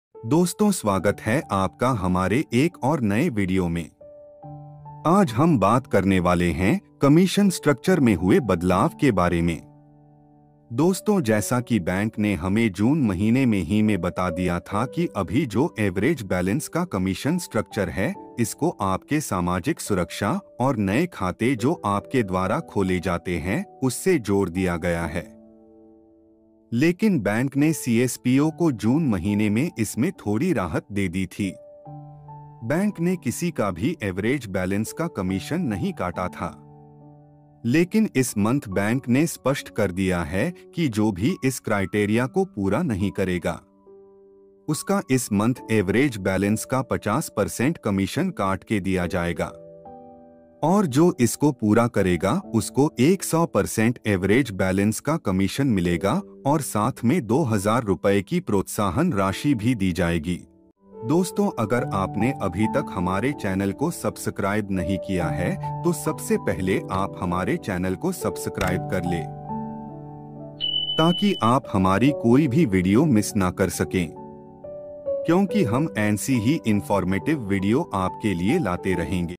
Most Important Update।। इस मंथ का काट के मिलेगा कमीशन।। sbi csp new update।।
is month bank kat ke dega average balance ka comission agar target complete nahi Kiya to